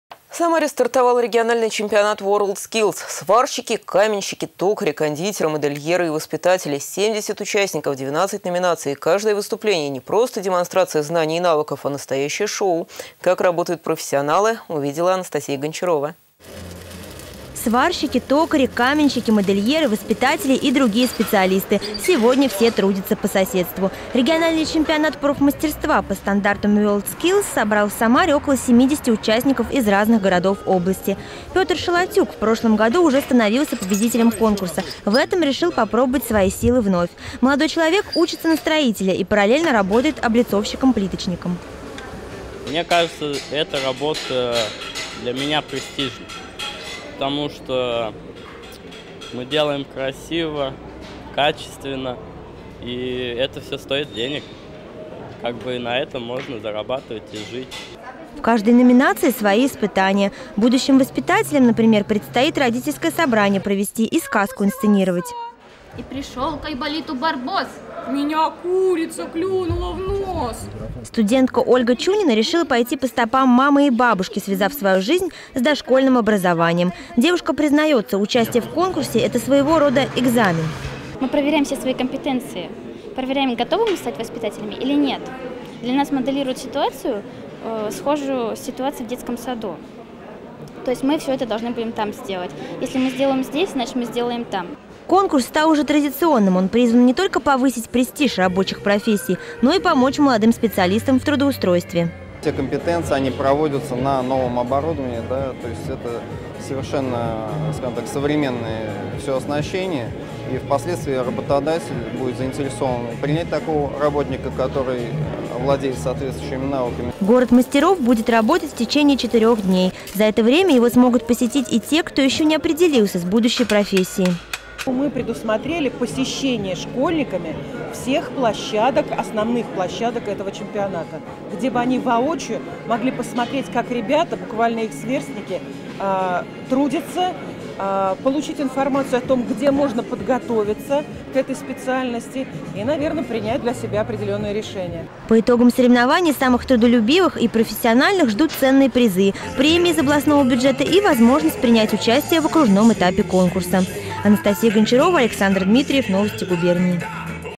В Самаре стартовал региональный чемпионат WorldSkills
В Самаре стартовал региональный чемпионат WorldSkills. Сварщики, каменщики, токари, кондитеры, модельеры и воспитатели. 70 участников, 12 номинаций и каждое выступление - не просто демонстрация знаний и навыков, а настоящее шоу. Как работают профессионалы, узнала наша съемочная группа.